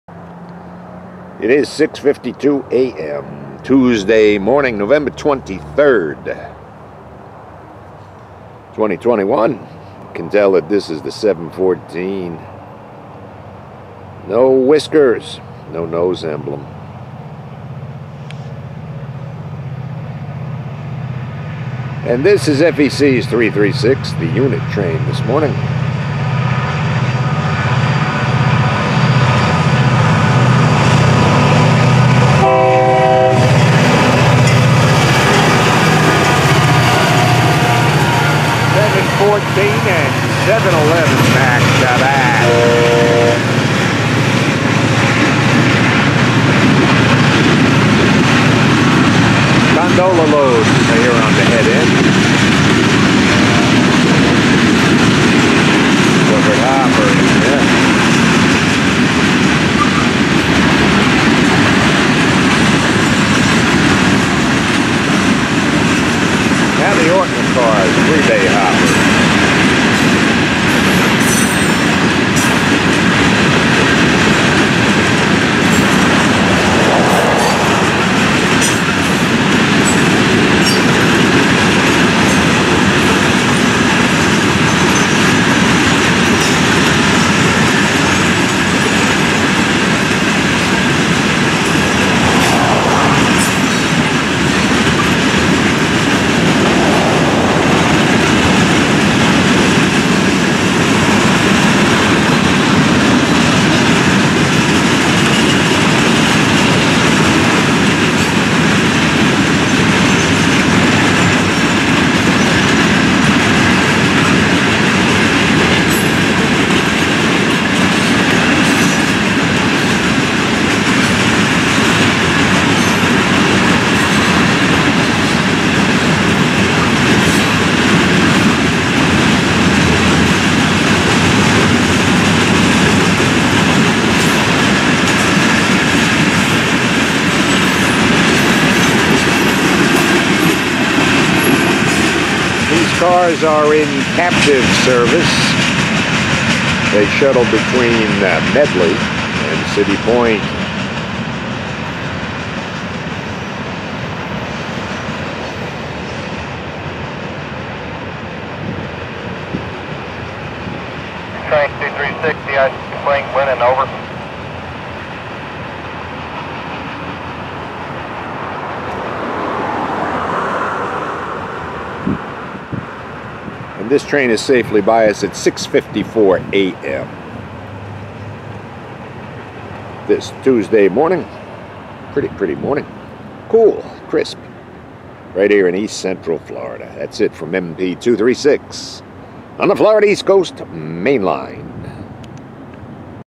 FEC336 Nov 23, 2021
714,711 back-to-back with the unit train.